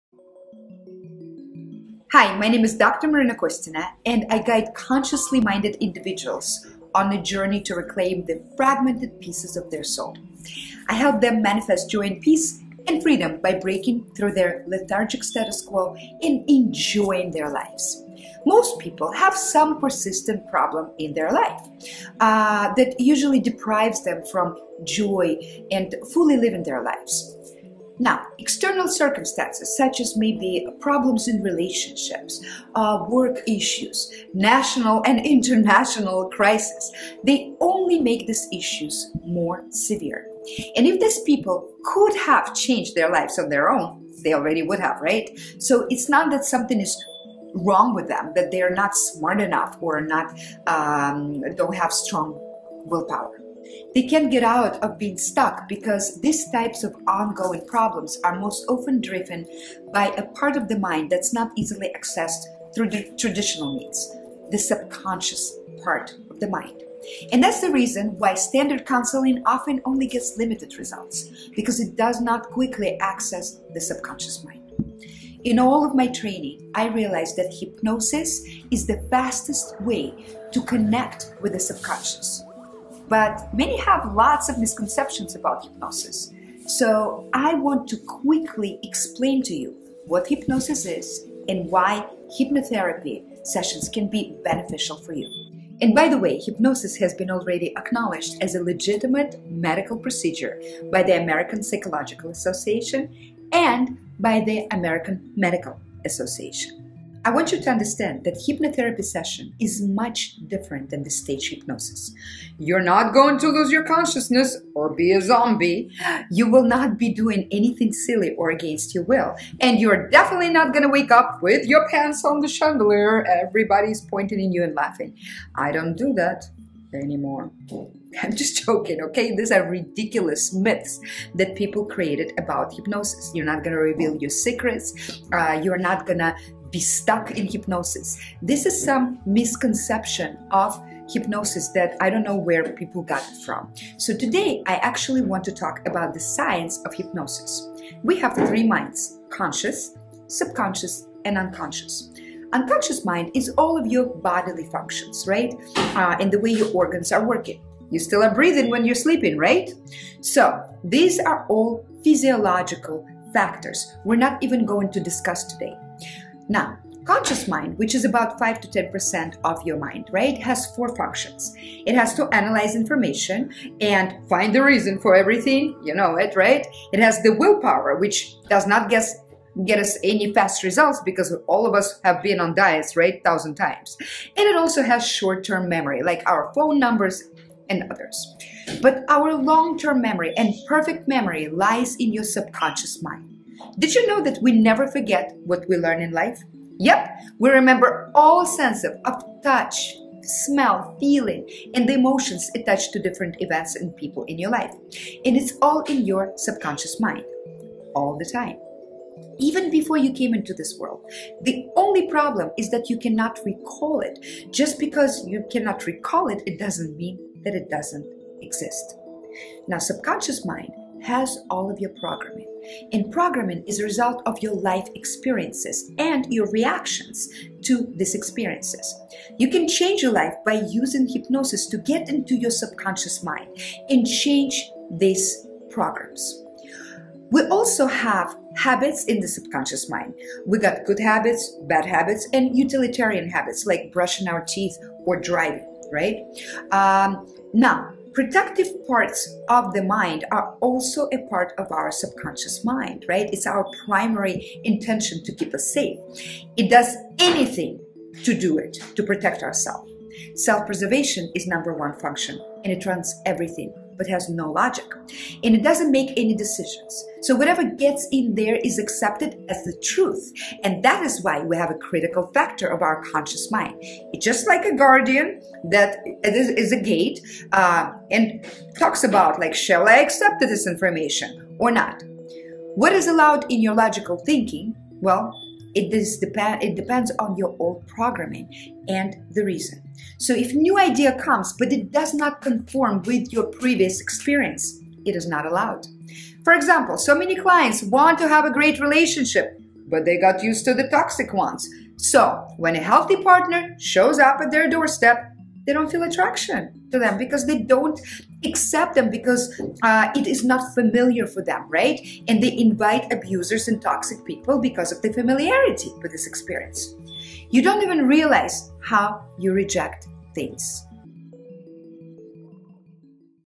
What is REALLY Hypnotherapy Session?
This video provides brief overview of hypnotherapy and explains how it works as well as demonstrates its benefits. Check out our virtual and face to face sessions and watch our clients' testimonials